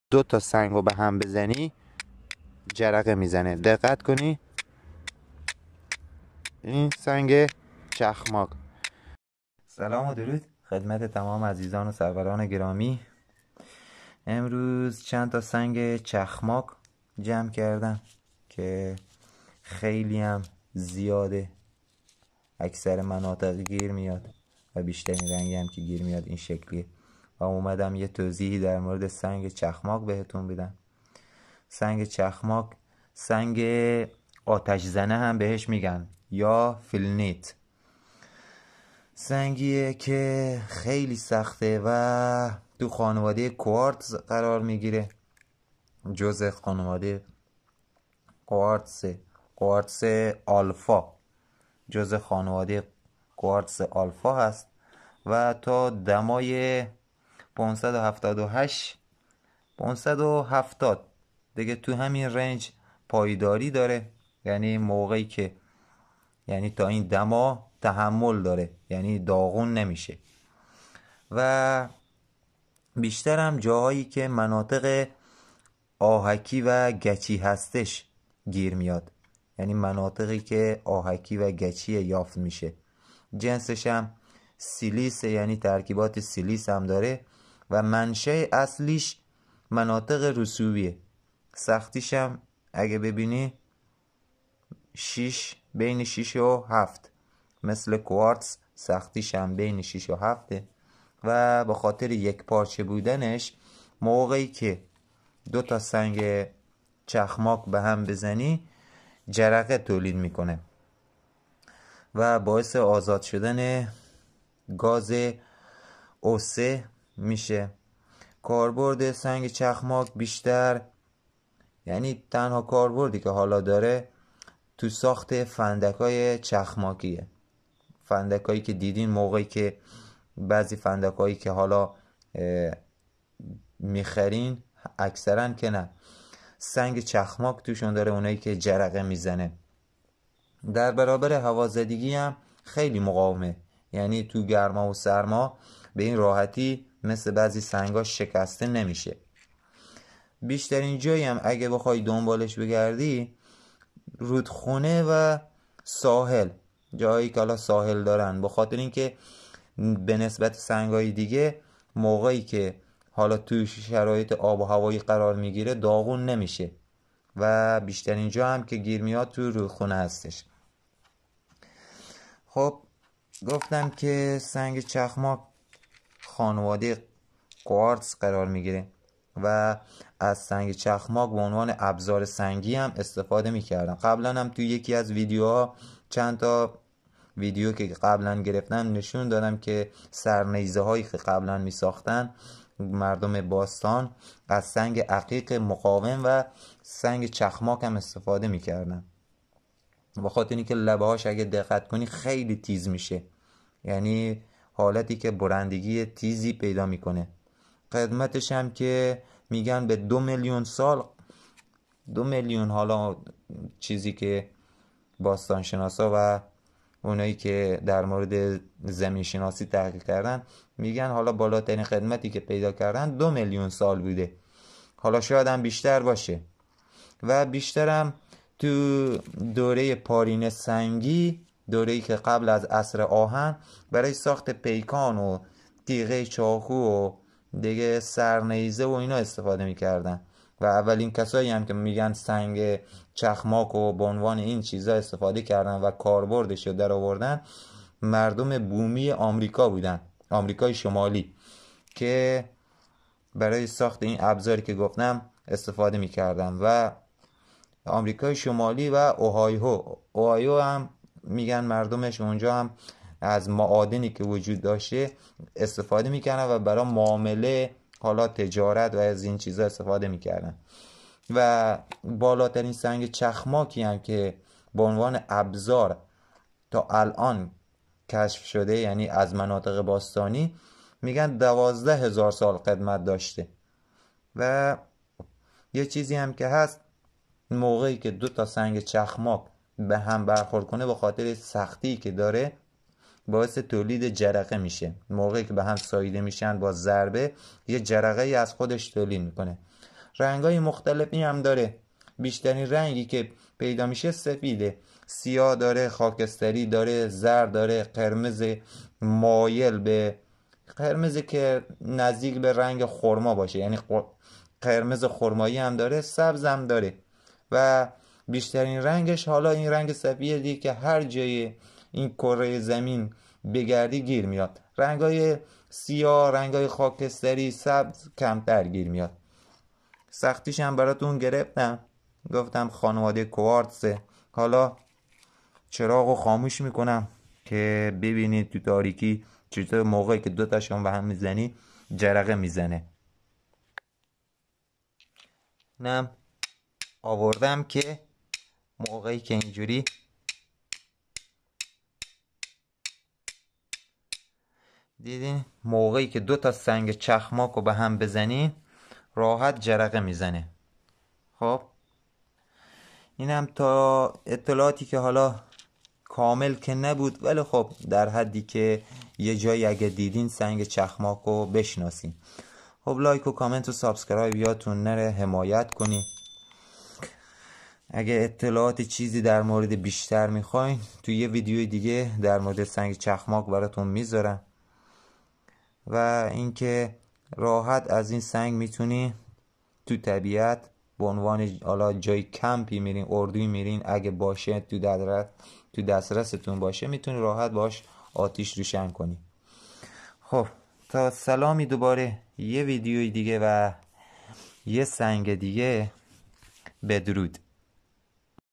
آموزش شناخت سنگ چخماق.تست سختی.kavoshminab.049
چنل کاوش میناب تو این ویدئو درمورد سنگ چخماق اطلاعات جامع داده. آموزش شناخت سنگ چخماق. سنگ چخماق جز خانواده کوارتز هستش و تا دمای 573درجه تحمل و پایداری داره. این سنگ کاربرد فراوانی داره. که یکی از کاربردهای آن درساخت فندک میباشد. ودر زمان باستان از این سنگ برای روشن کردن آتش استفاده می‌کردند. بهترین مکان برای پیداکردن سنگ چخماق رودخانه و کنار ساحل میباشد. چنل آموزش تست سنگهای قیمتی و چگونگی شناخت شهاب سنگ وبهترین مکان برای پیداکردن دفینه. کاوش میناب. Kavoshminab